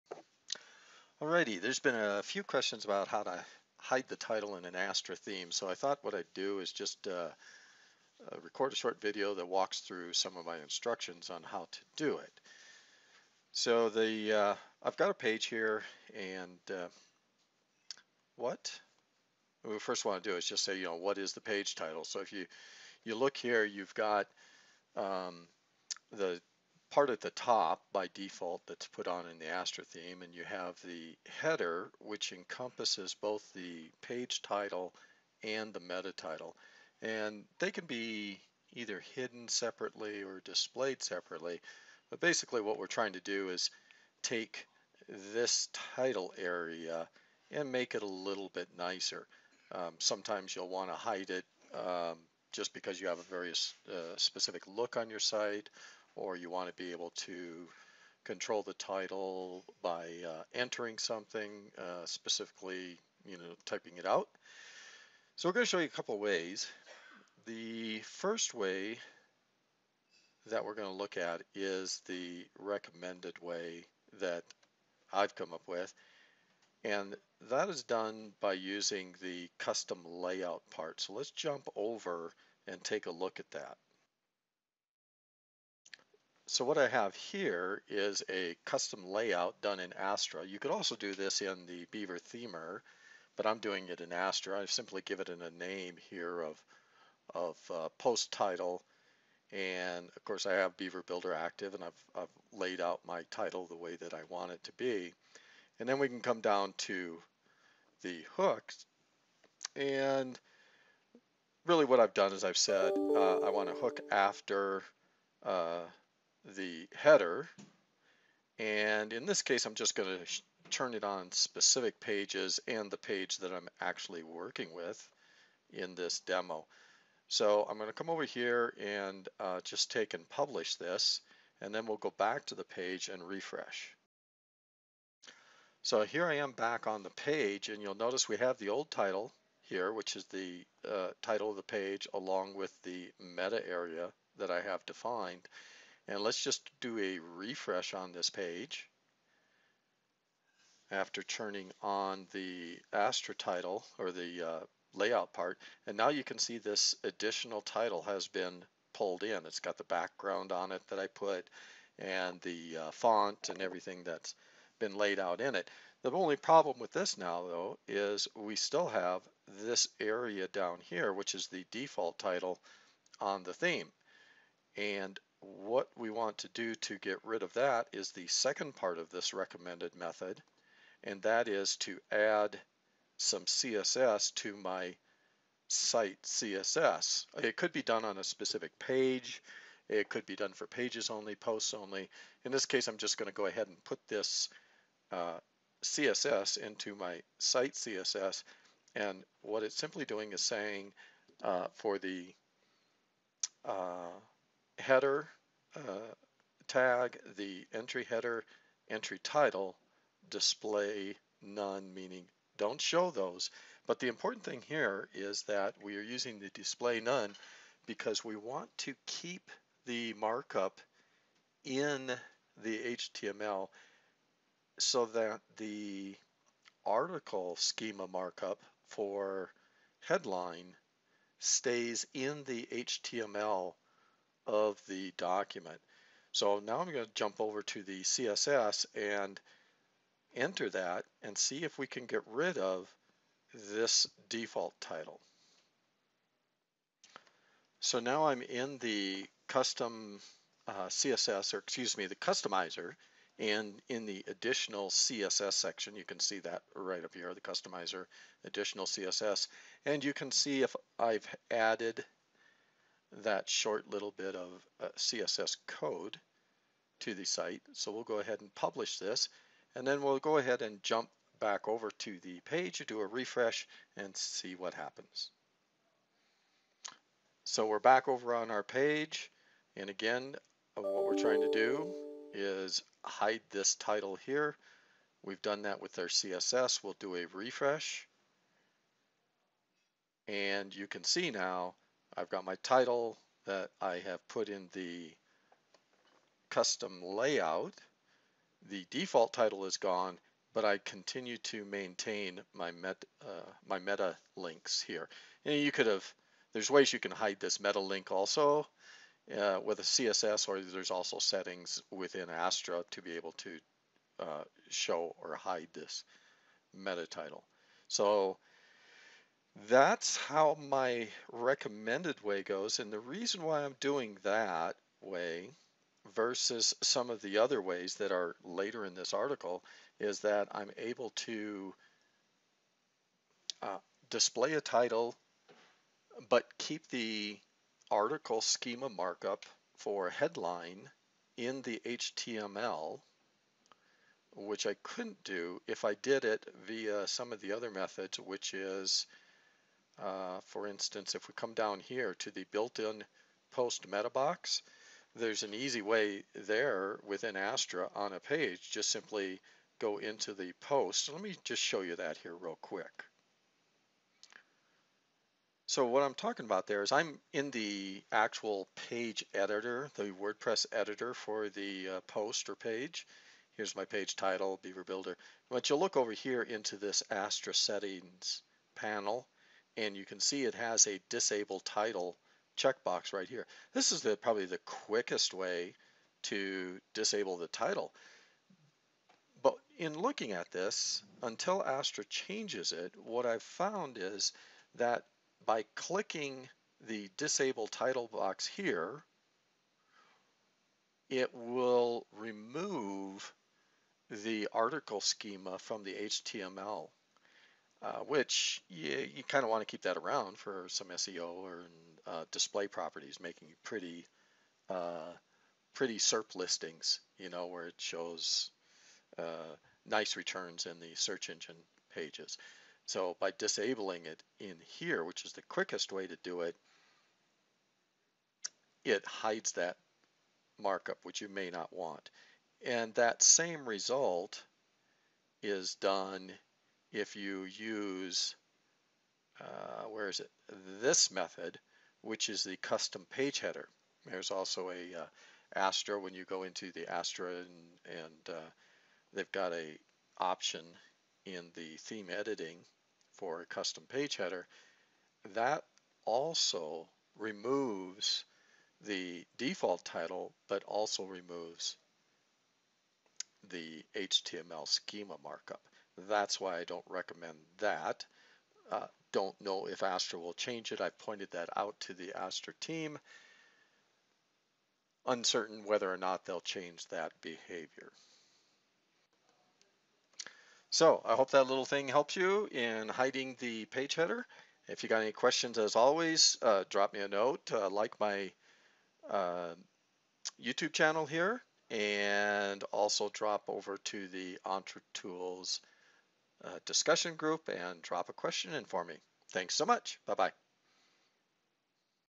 Hiding the Title in Astra
This is a walkthrough of how to hide a page  or post title in Wordpress with the Astra theme and BeaverBuilder page builder.